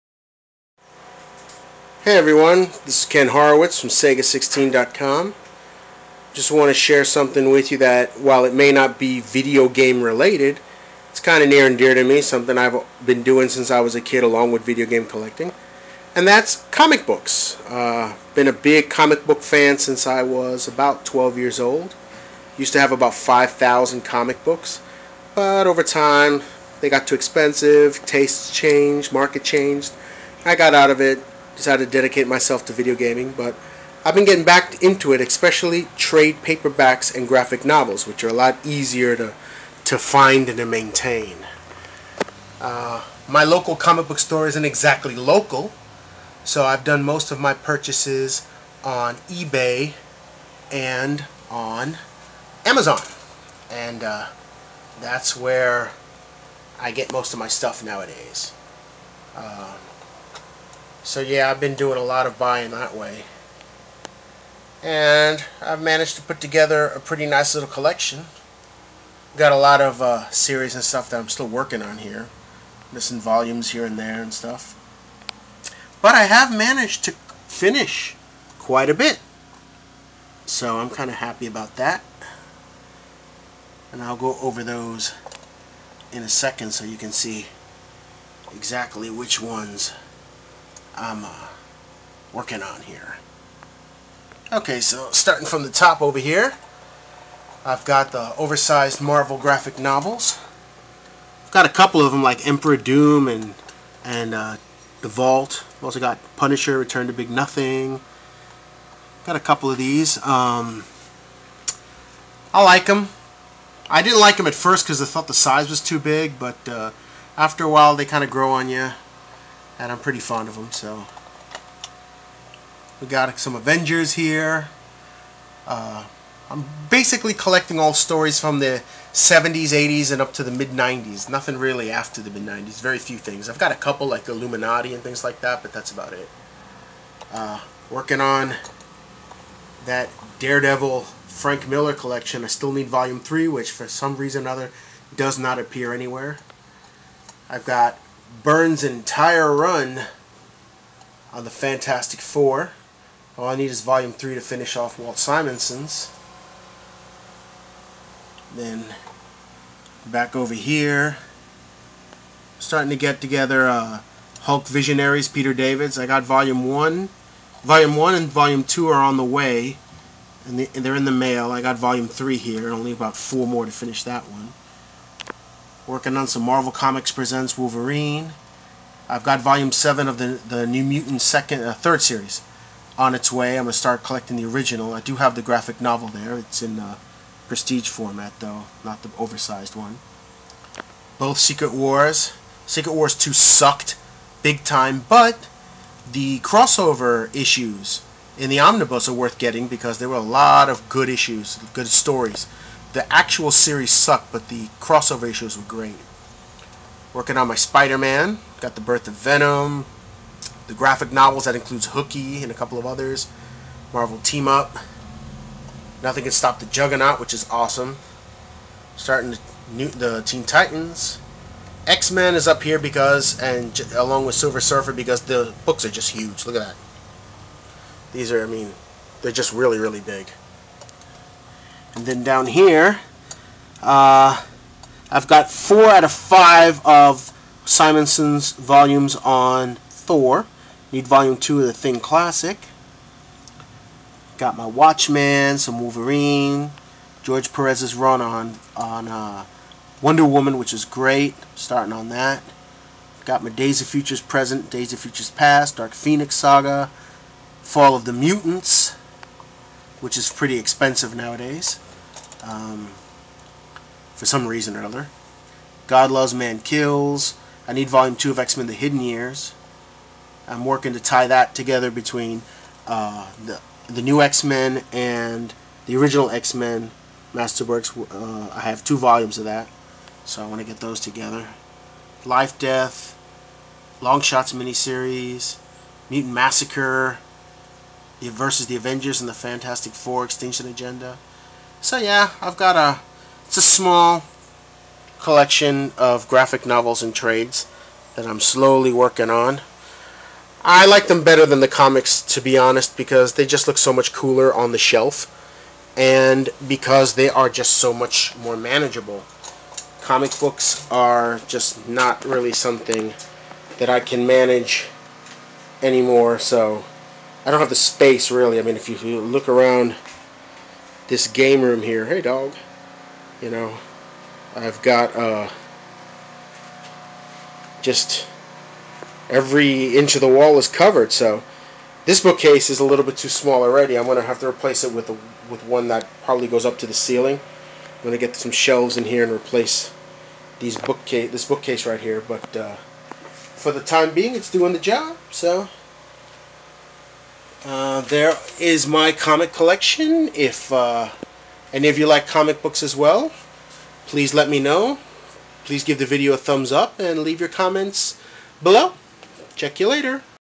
Melf's Comic Graphic Novel & TPB Collection
Something a little different! Forgive the poor cell phone quality. I just wanted to share what is another favorite pastime of mine: comic books! I like graphic novels and trade paperbacks because they're much easier to manage and just look so cool on the shelf. I mostly collect stuff from between 1975 and 1995, which is the era where I did most of my heavy collecting.